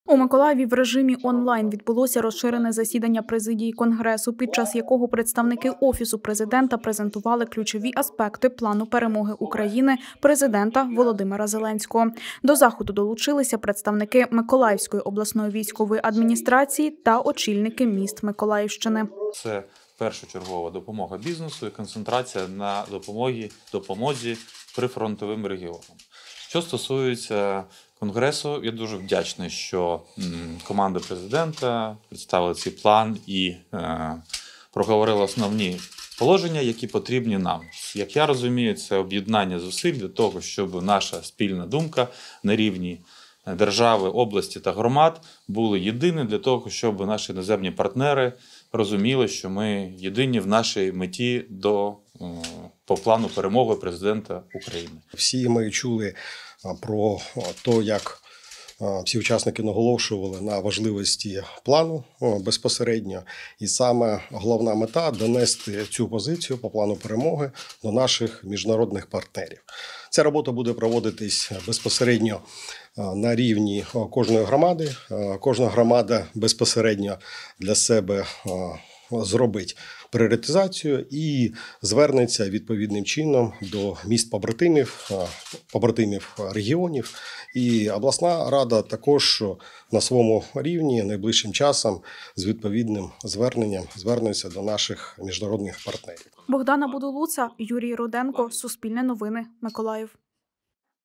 Миколаївські посадовці долучилися до засідання президії Конгресу місцевих та регіональних влад
24 жовтня в режимі "онлайн" відбулося розширене засідання президії Конгресу місцевих та регіональних влад. Представники Офісу Президента презентували ключові аспекти плану перемоги України, запропонованого Володимиром Зеленським. До заходу долучилися представники Миколаївської обласної військової адміністрації та очільники населених пунктів регіону.